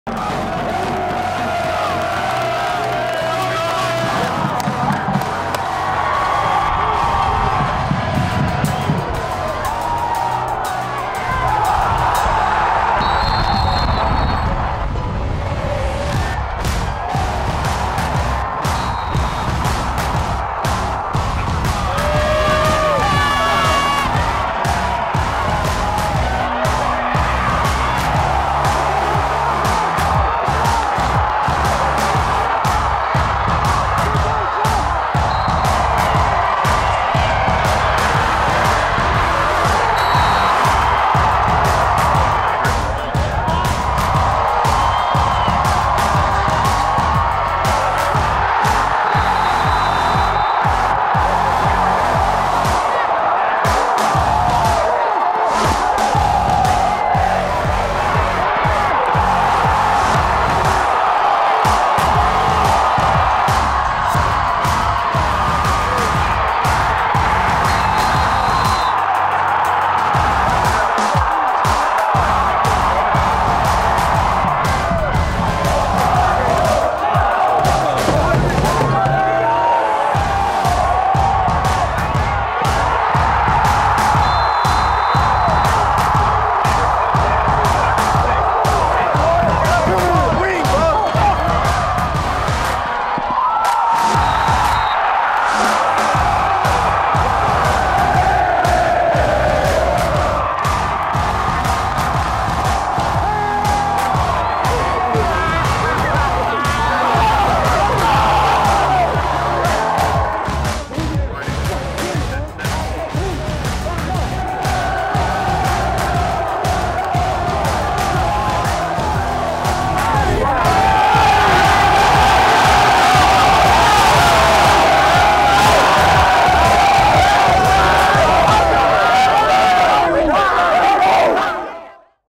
Full highlights: De Smet wins 2019 Missouri Class 6 State Championship
The Spartans capped off their undefeated 14-0 season with a state championship over Joplin in Columbia winning 35-20.